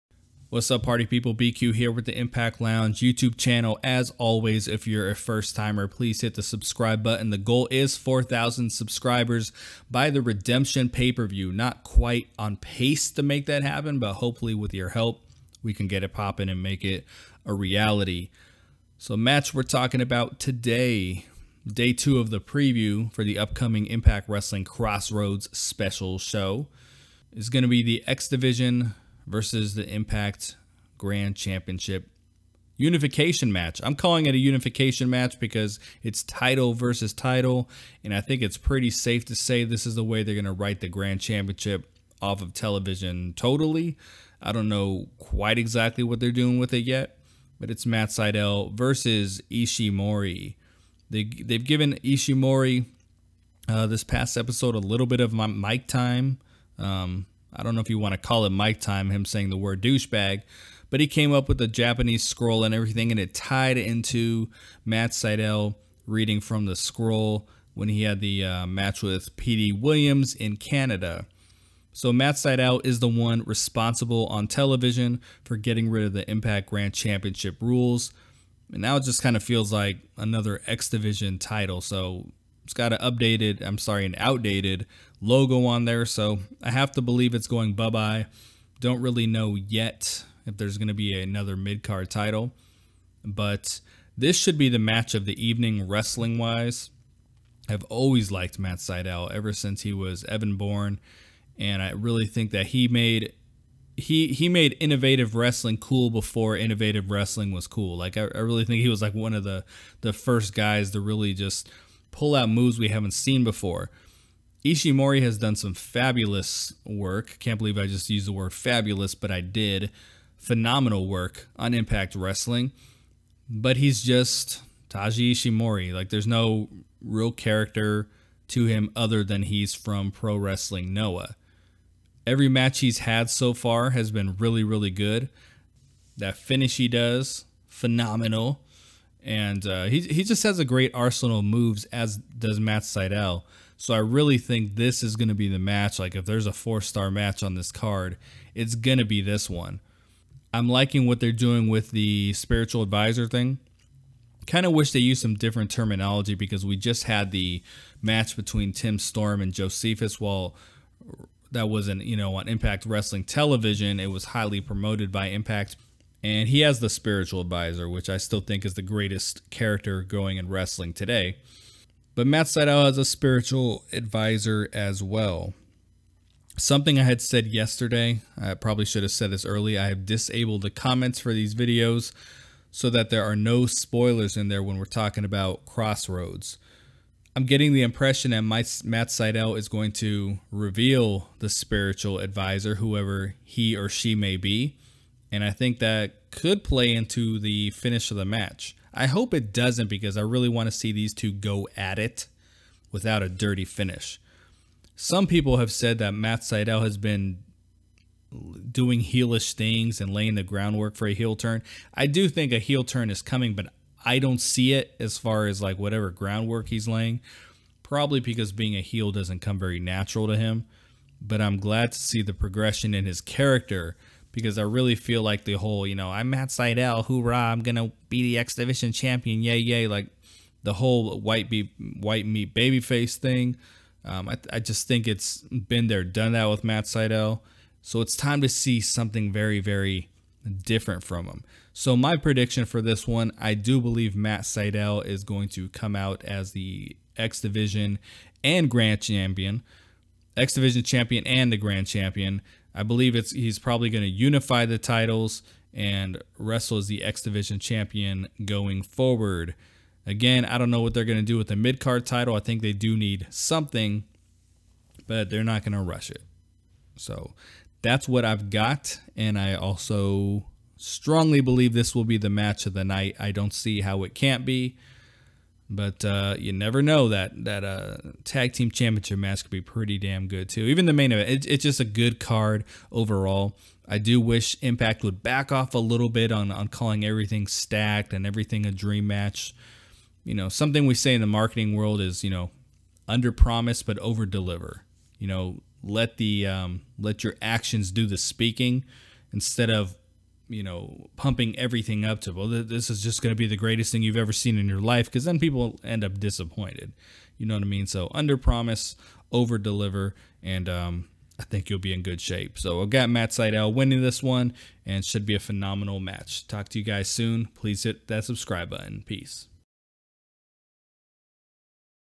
IMPACT Crossroads - Taiji Ishimori vs Matt Sydal (Preview & Prediction)
Who will leave the double champion when Taiji Ishimori (X-Division Champions) takes on Matt Sydal (Impact Grand Champion)? What role will the spiritual advisor play?